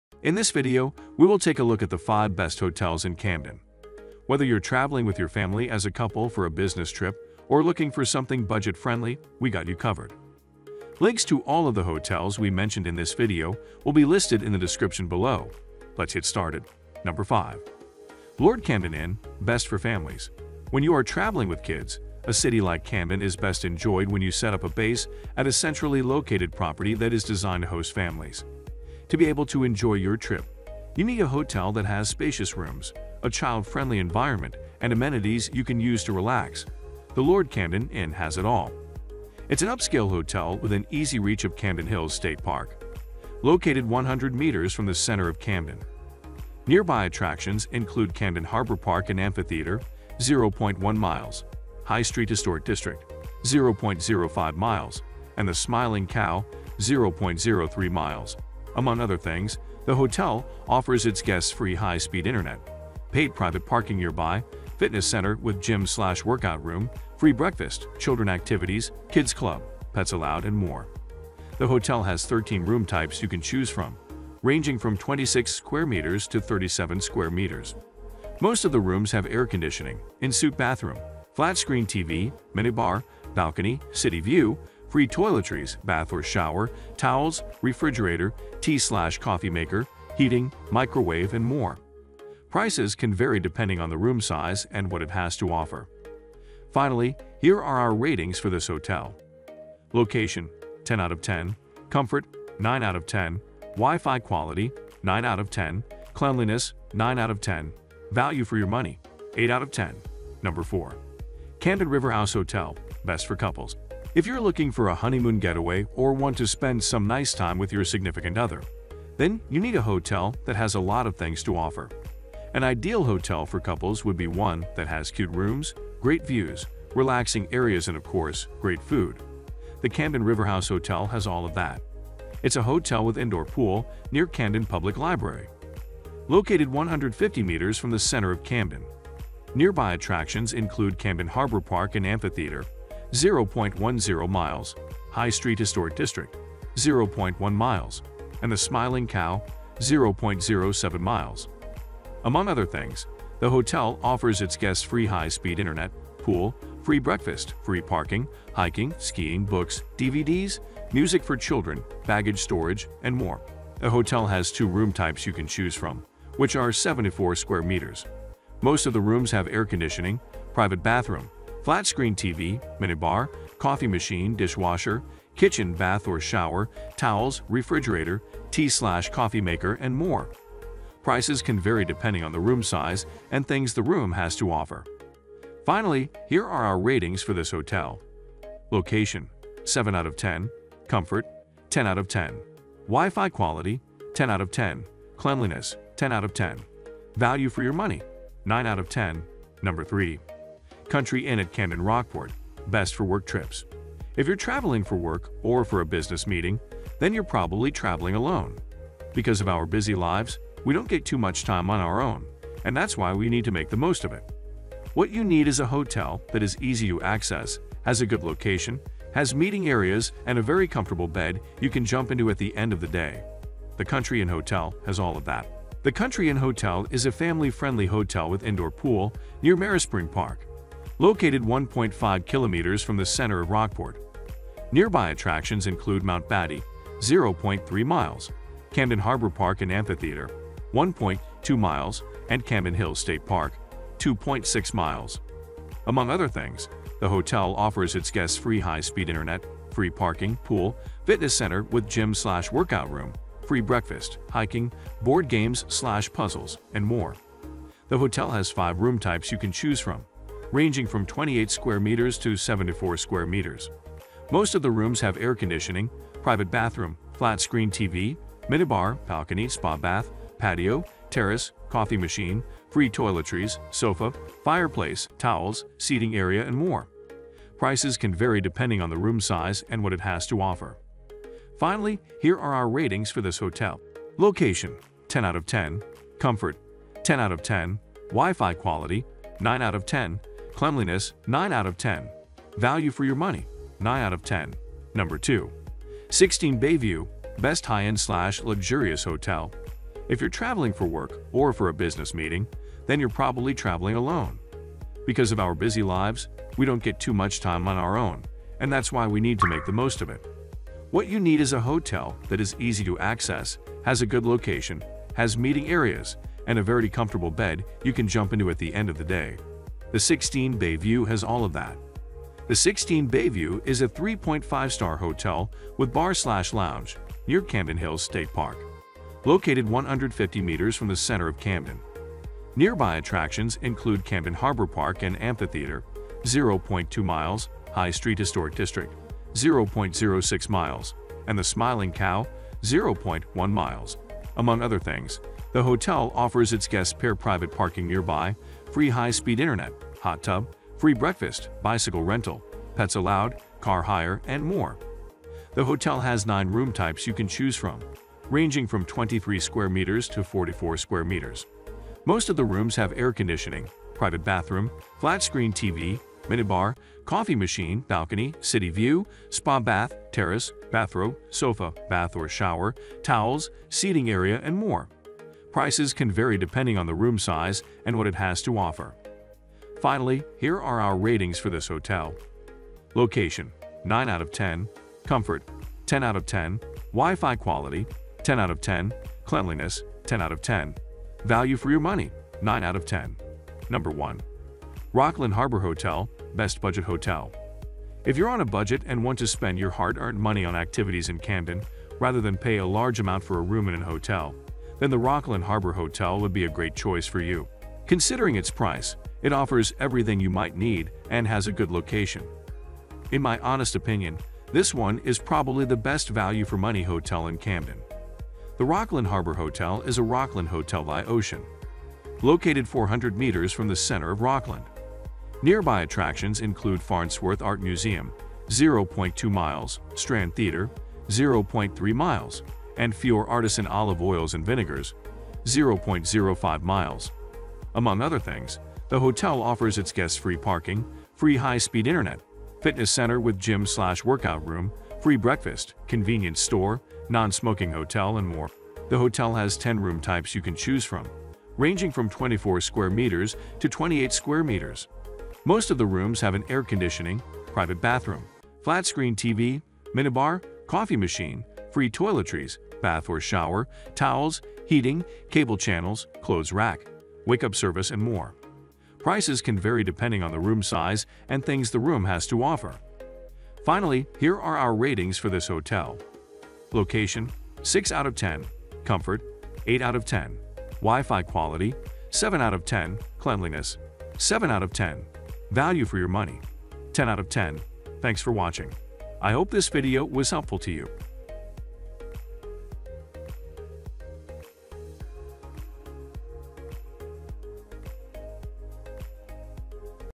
Best Hotels In Camden - For Families, Couples, Work Trips, Luxury & Budget
► Links to the best hotels in Camden we mentioned in this video:

3. Country Inn at Camden Rockport

In this video, we listed the top 5 best hotels in Camden. I made this list based on my personal opinion, and i tried to list them based on their price, quality, location and more. If you want to see the price and find out more information about these hotels, you can check out the links above.